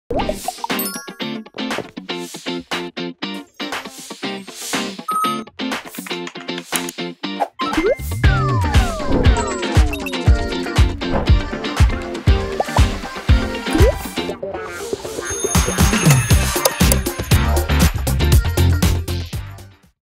공직사회 조직문화 혁신 홍보동영상
2021년 정부혁신의 일환인 " 공직사회 조직문화 혁신"  확산을 위해 행정안전부에서 제작한 홍보동영상